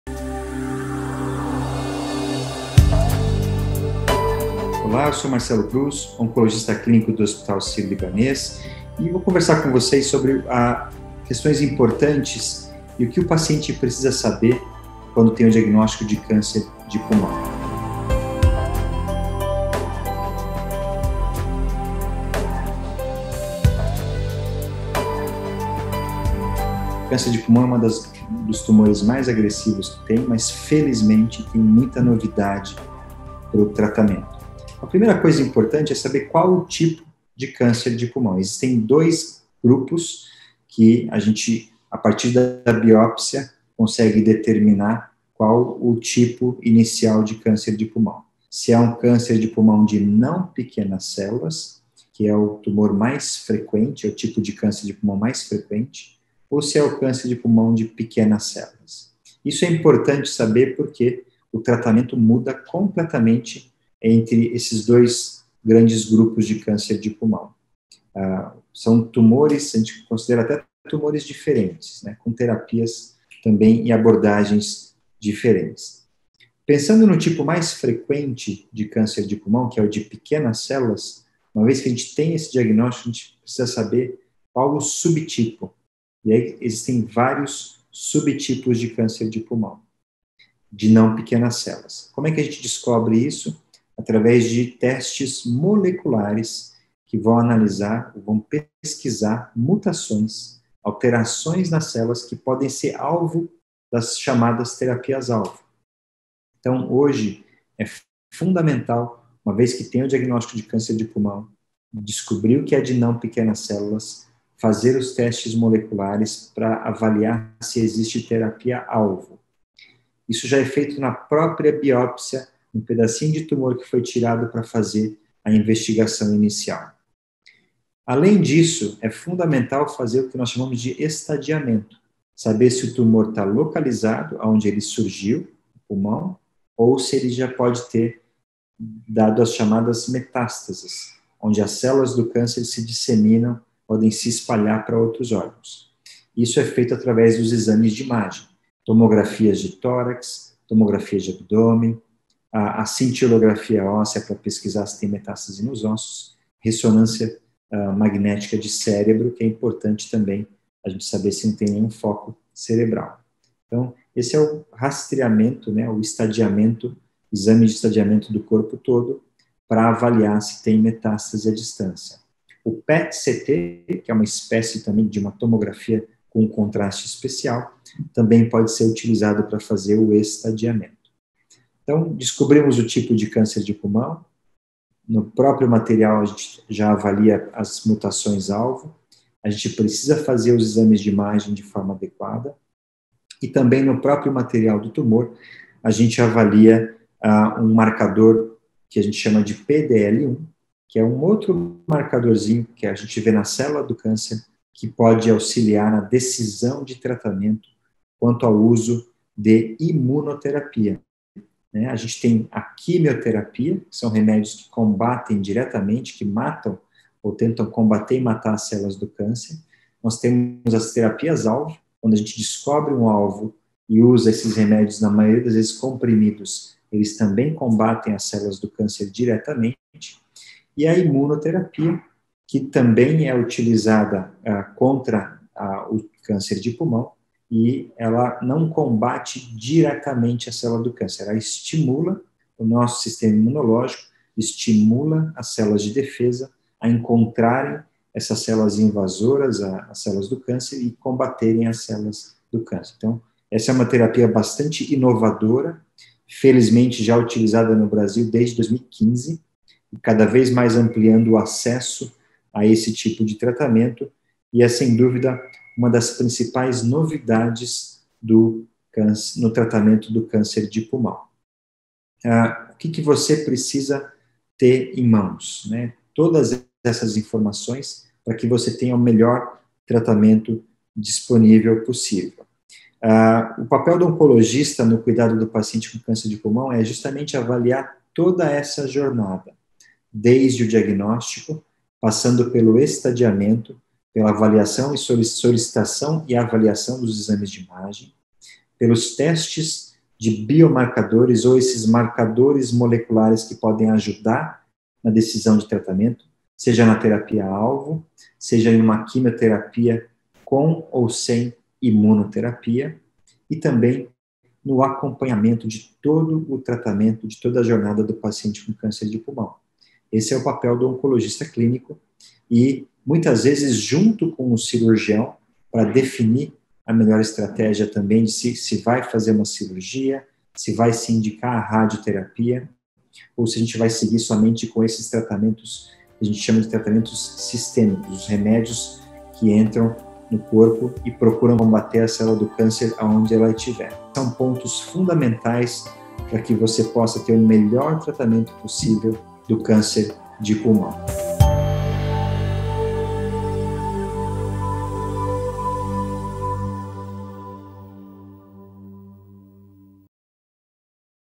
Tudo o que você precisa saber sobre o câncer de pulmão - Marcelo Cruz
Apesar de ser um tipo de tumor agressivo, há muitas novidades em tratamento para o câncer de pulmão. Neste vídeo, o oncologista do hospital Sírio Libanês, Marcelo Cruz, comenta algumas dessas novidades importantes e fala um pouco mais sobre informações fundamentais para o paciente com a doença. Assista!